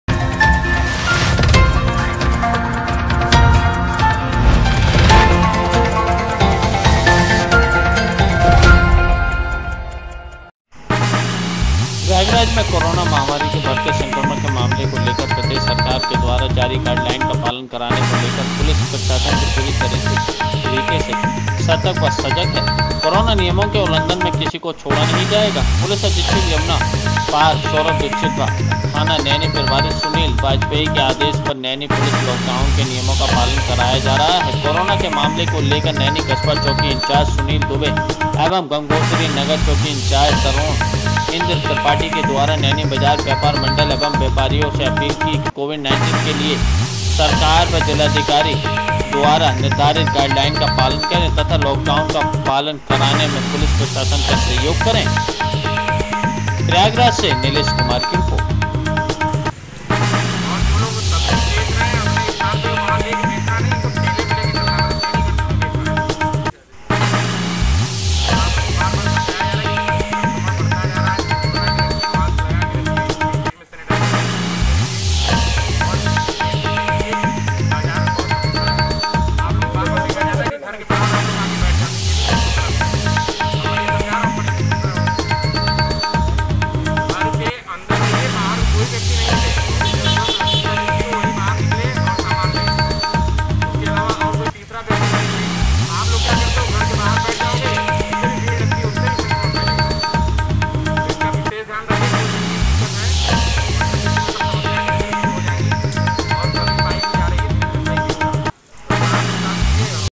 प्रयागराज में कोरोना महामारी के बढ़ते संक्रमण के मामले को लेकर प्रदेश सरकार के द्वारा जारी
प्रयागराज में कोरोना महामारी के बढ़ते संक्रमण  के मामले को लेकर प्रदेश सरकार के द्वारा जारी गाइडलाइंस का पालन कराने को लेकर पुलिस प्रशासन पूरी तरीके से सतर्क व सजग है कोरोना नियमों के उल्लंघन में किसी को छोड़ा नहीं जाएगा
 पुलिस अधीक्षक यमुना पार सौरभ दीक्षित व थाना नैनी प्रभारी सुनील बाजपेई के आदेश पर नैनी पुलिस लॉकडाउन के नियमों का पालन कराया जा रहा है कोरोना के मामले को लेकर नैनी कस्बा चौकी इंचार्ज सुनील दुबे एवं गंगोत्री नगर चौकी इंचार्ज तरुणइंद्र त्रिपाठी के द्वारा नैनी बाजार व्यापार मंडल एवं व्यापारियों से अपील की कोविड-19 के लिए सरकार व जिलाधिकारी द्वारा निर्धारित गाइडलाइंस का पालन करें तथा लॉक डाउन का पालन कराने में पुलिस प्रशासन का सहयोग करें
 आपके बीच से लगातार देखने को मिल रहा है कि नैनी बाजार एवं आसपास की एरिया में लॉकडाउन के दौरान भी दुकान खोल कर बिक्री की जा रही है सरकार ने लॉकडाउन इसलिए लगाया है कि आप सभी का जीवन बचे और कोरोना का चैन टूट सके और सबका परिवार सुरक्षित रहे व्यापार मंडल एवं व्यापारियों से अपील करता हूं नैनी बाजार नियमानुसार बंद रखें तथा बाजार बंद करके पुलिस प्रशासन का सहयोग करें

Prime 18 news से  संवाददाता निलेश कुमार क्राइम रिपोर्टर प्रयागराज